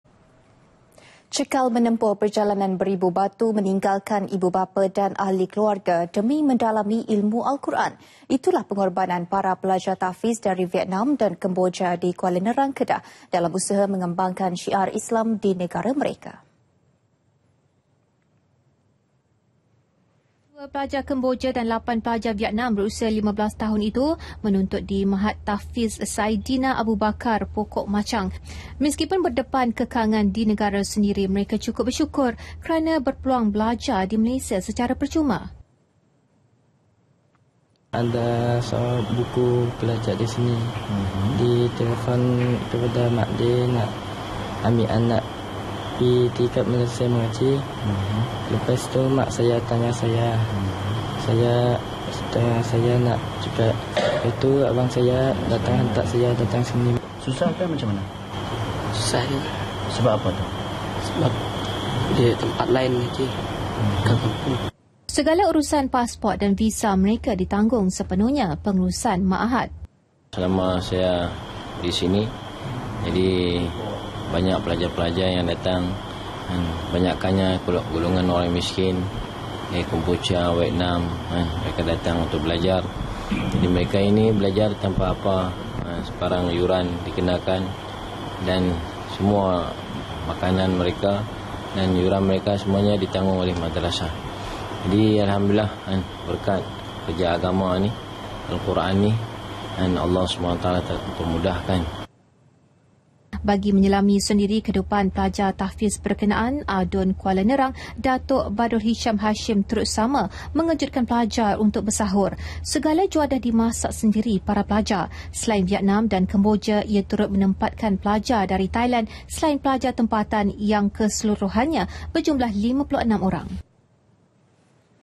PENGORBANAN PELAJAR TAHFIZ VIETNAM & KEMBOJA [25 JUN 2015]
Cekal menempuh perjalanan beribu batu meninggalkan ibu bapa dan ahli keluarga, demi mendalami ilmu Al-Quran.

Itulah pengorbanan para pelajar tahfiz dari Vietnam dan Kemboja di Kuala Nerang, Kedah... dalam usaha mengembangkan syiar Islam di negara mereka.